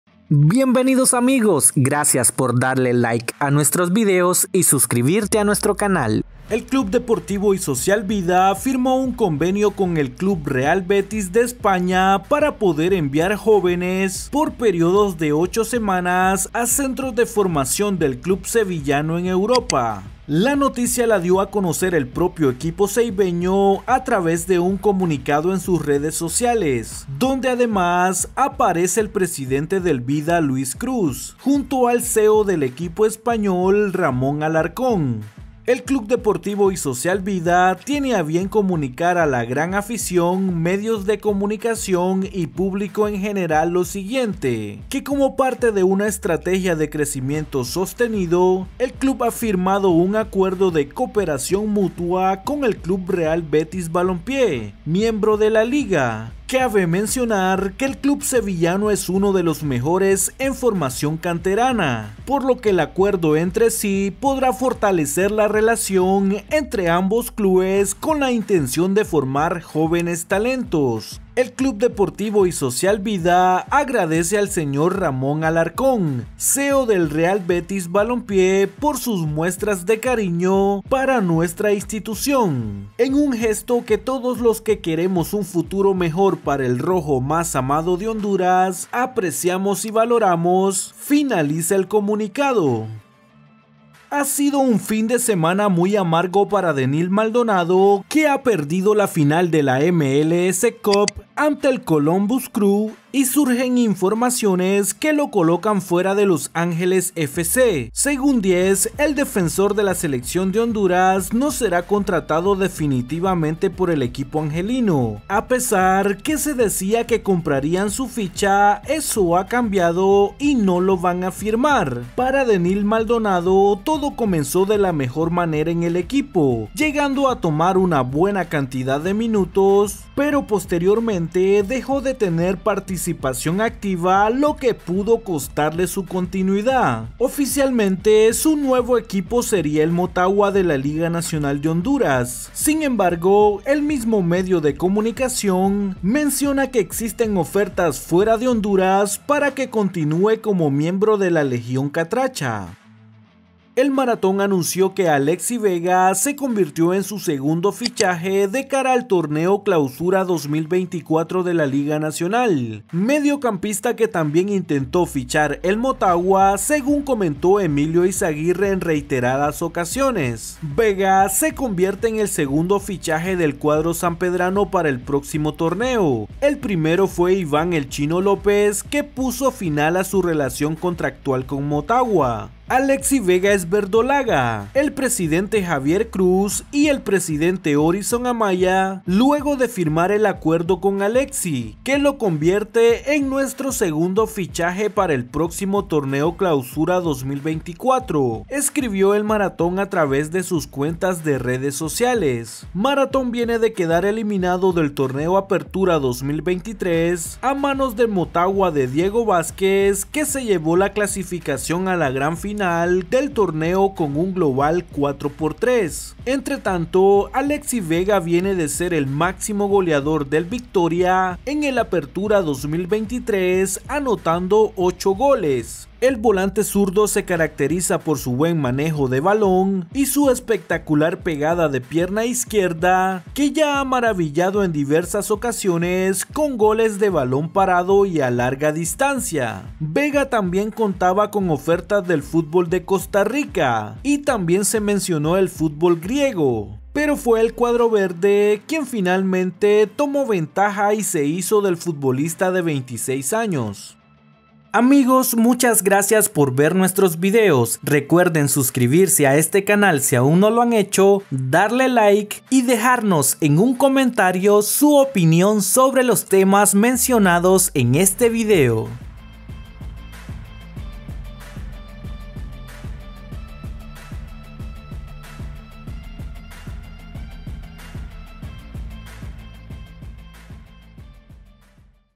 Vida Firma Importante Acuerdo Con El Real Betis De España | Denil Maldonado Fuera De Los Angeles FC
Vida Firma Importante Acuerdo Con El Real Betis De España || Denil Maldonado Fuera De Los Angeles FC || Oficial Alexy Vega Al Marathon